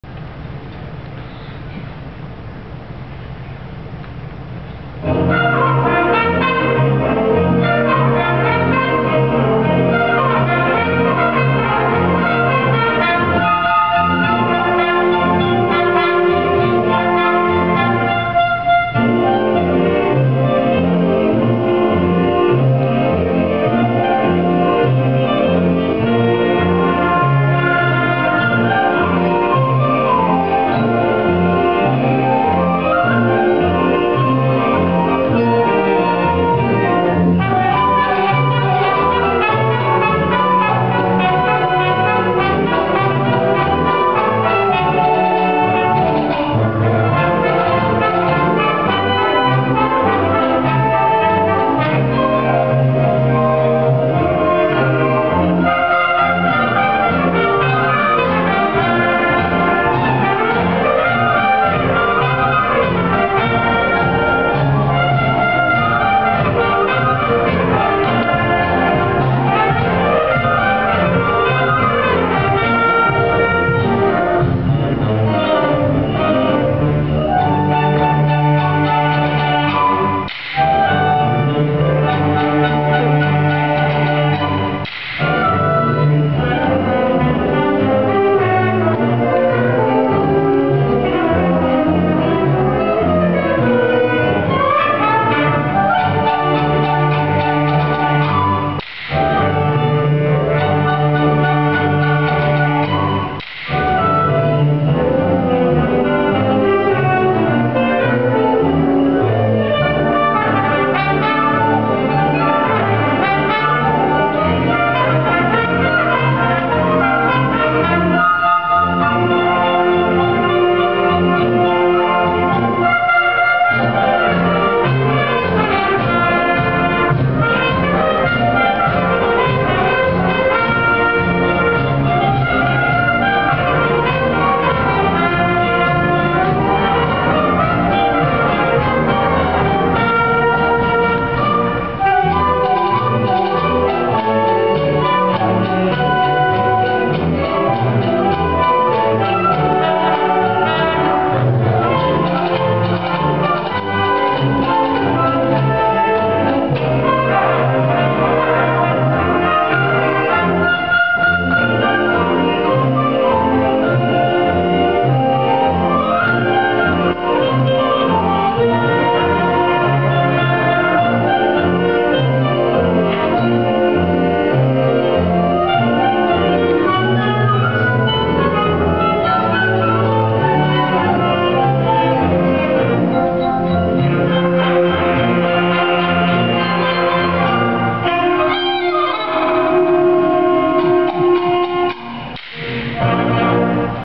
Nathan Christmas Concert
December 11, 2009
Bethesda Christian School, Junior Year